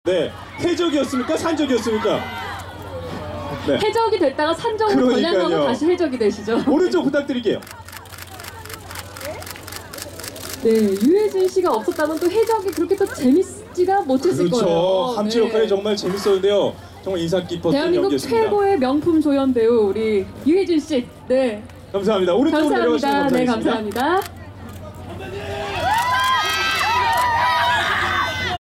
2014 대종상영화제 레드카펫. 유해진 남우조연상 수상
2014년 11월 21일(금) 여의도 KBS홀에서 열린 51회 대종상영화제 식전 레드카펫 현장.
최우수작품상 명량 김한민/ 감독상 끝까지 간다 김성훈/ 남우주연상 명량 최민식/ 여우주연상 해적 손예진/ 남우조연상 해적 유해진/여우조연상 변호인 김영애/ 신인남자배우상 해무 박유천/ 신인여자배우상 인간중독 임지연/ 신인감독상 변호인 양우석/ 공로상 정진우